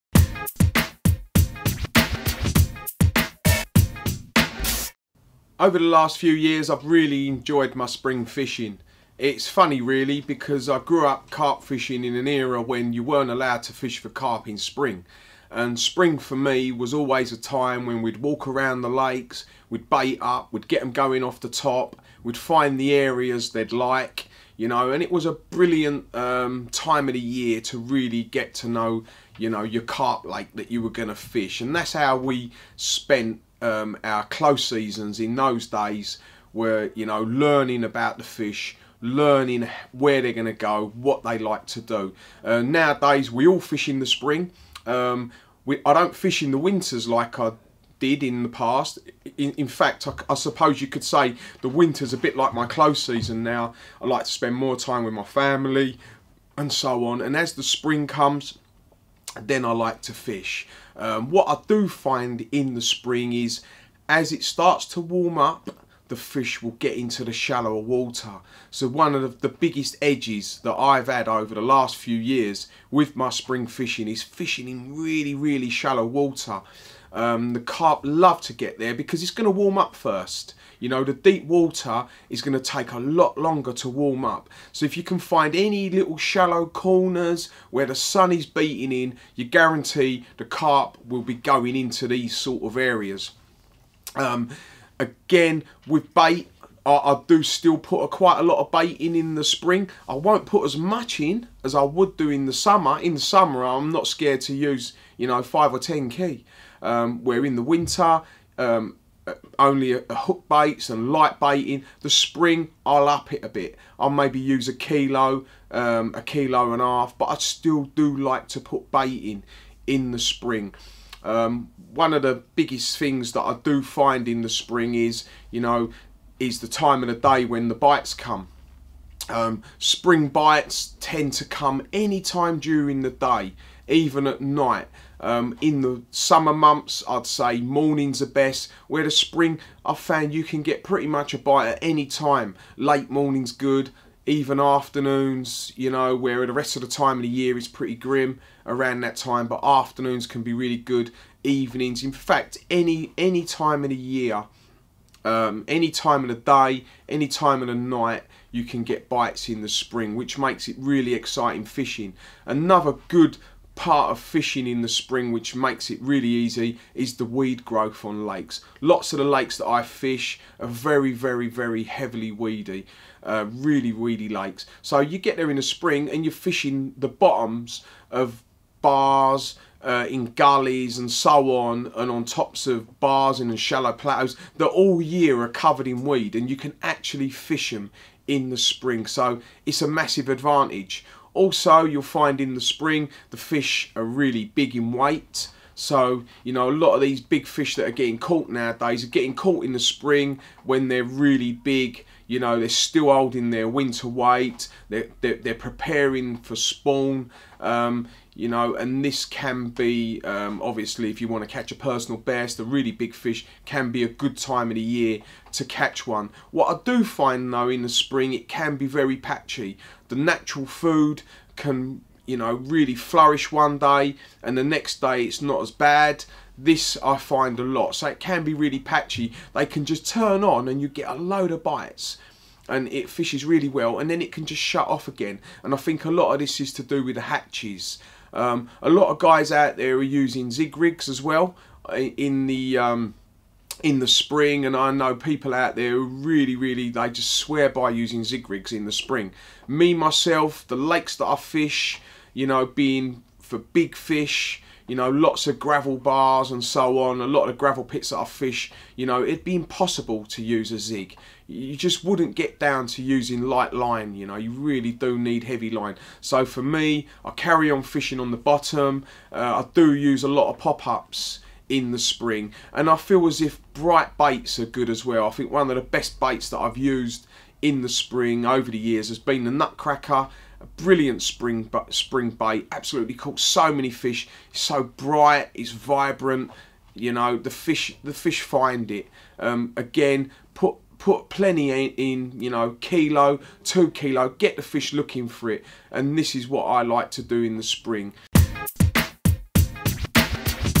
Spring tips with Terry Dempsey
Urban bait's main man and Carp fishing legend Terry Dempsey starts his series of Q&A videos with TackleBox TV . Tune in every friday for more hints and Tip from the man himself!